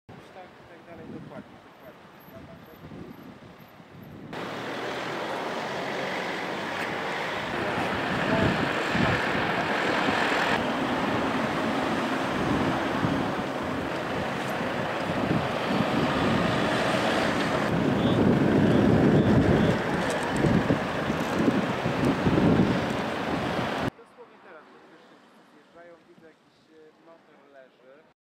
Piła: zdarzenie auta z motocyklem
Do zdarzenia drogowego doszło na skrzyżowaniu ul. Okrzei z ul. Sikorskiego. Kierowca samochodu potrącił motocyklistę.